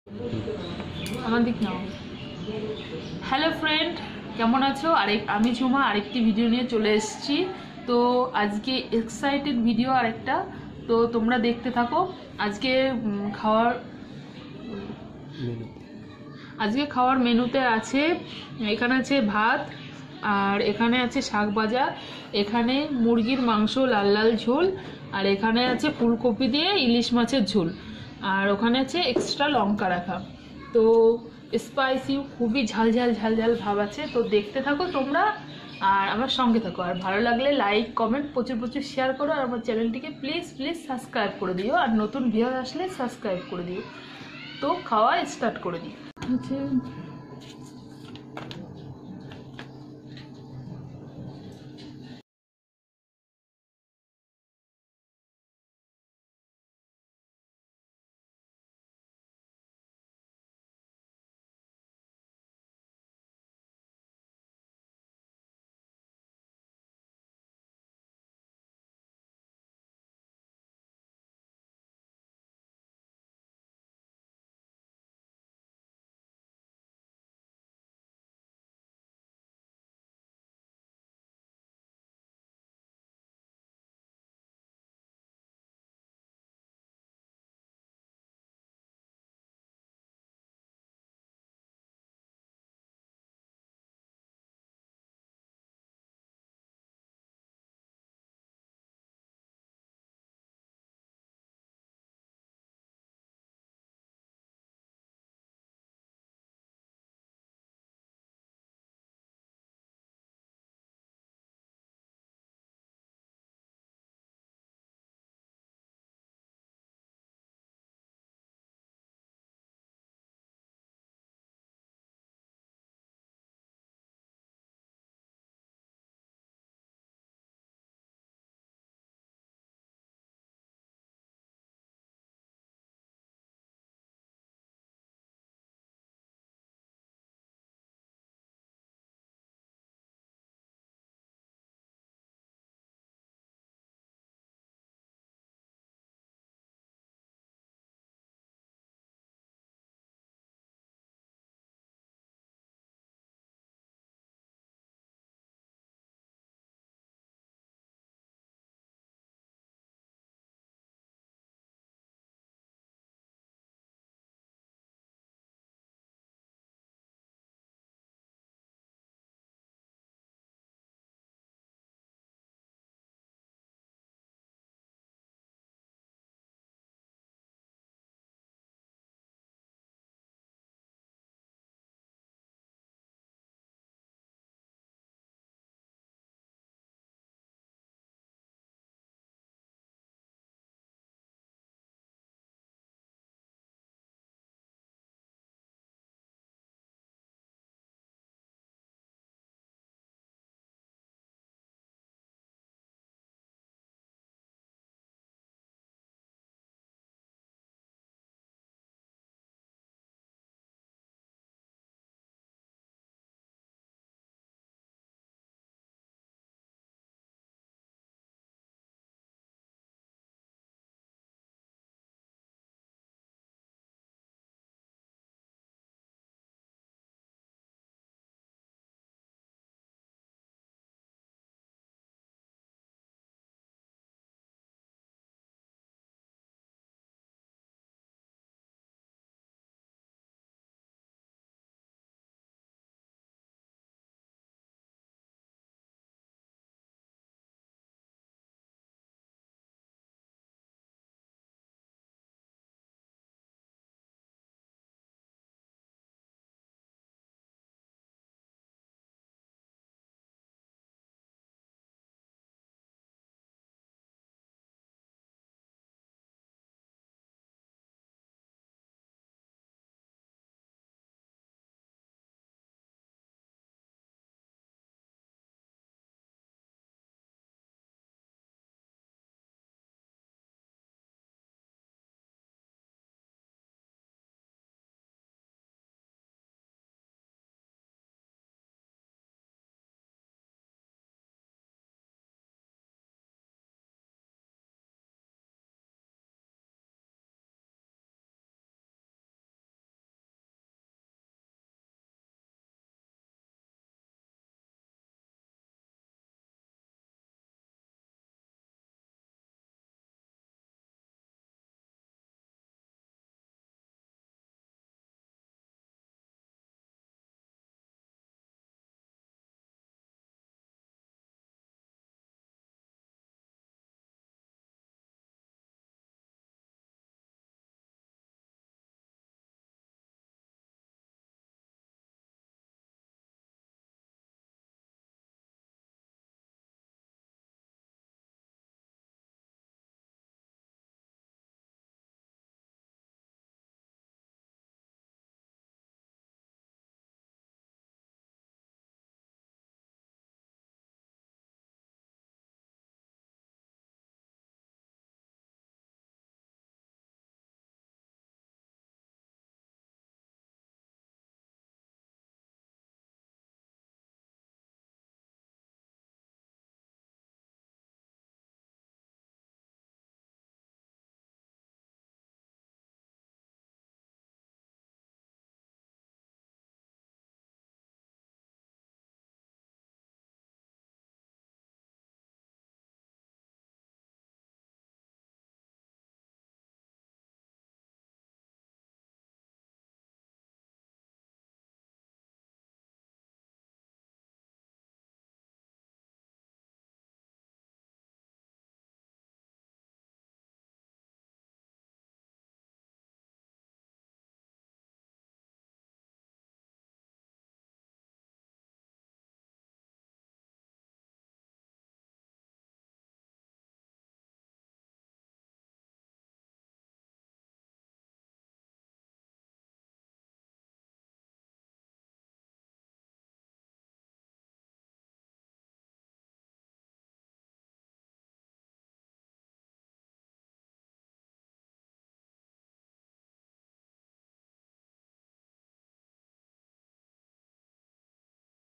Eating Show 🐔Rice with very spicy Chicken Curry ,vegetable,fish Curry
Eating Show 🐔Rice with very spicy Chicken Curry ,vegetable,fish Curry@eatingshowjhuma6652

Hi friends today I am going to eat verry delicious food